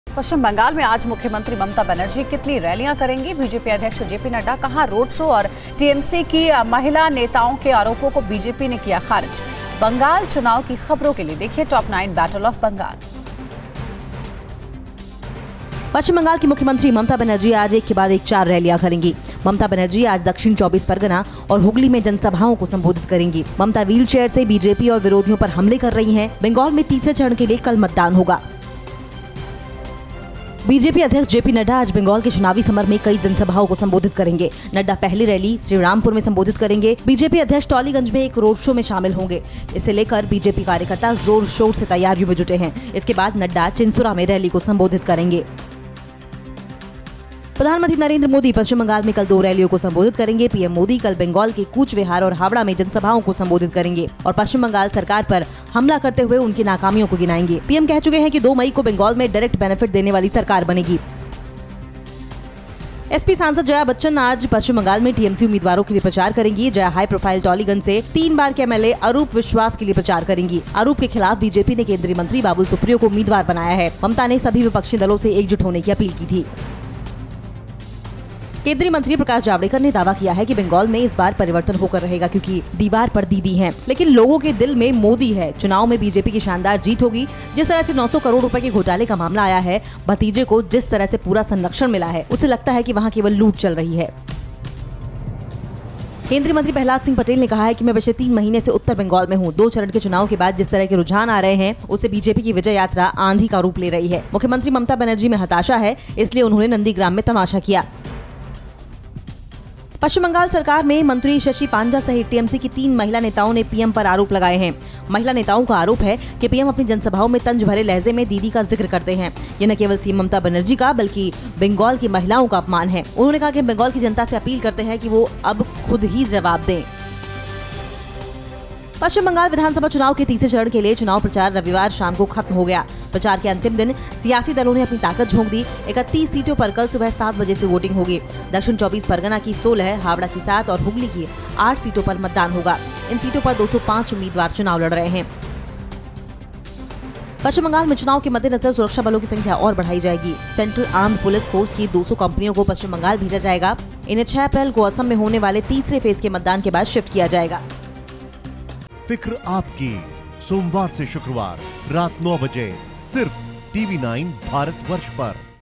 News Top 9 Battle of Bengal: आज Mamata Banerjee 4 रैलियां करेंगी
- पश्चिम बंगाल की मुख्यमंत्री ममता बनर्जी आज एक के बाद एक चार रैलियां करेंगी

- प्रधानमंत्री नरेंद्र मोदी पश्चिम बंगाल में कल दो रैलियों को संबोधित करेंगे

Credit